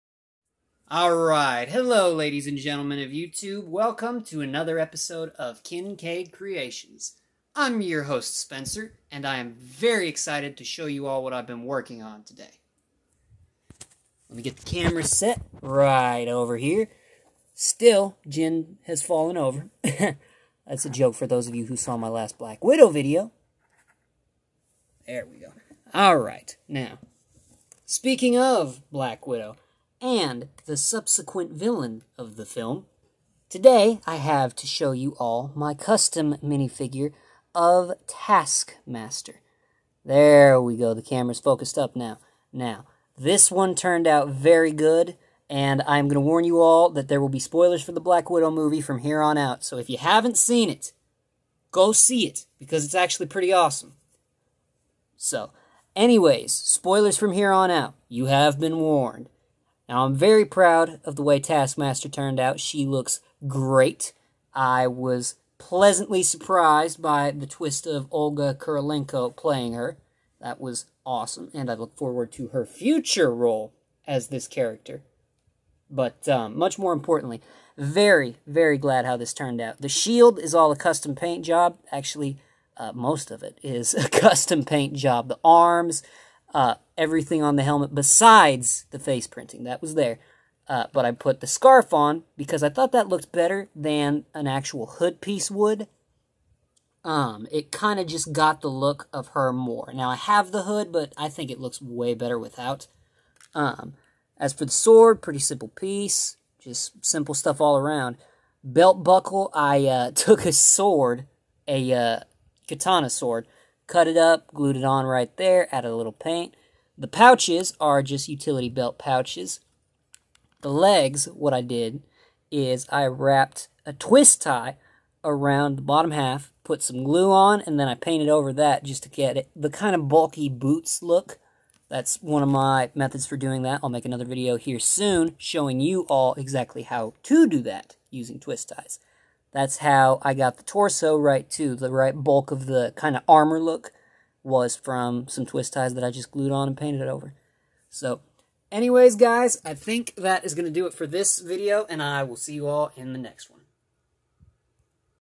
lego Taskmaster custom
Hello youtube, welcome to another episode of kincaid kreations, today I'm excited to show you all the custom minifigure I made of taskmaster from the new black widow movie. ALERT:

SPOILER WARNING:

Lol but seriously there are spoilers for the movie in this vid, anyway I'll see you in the video.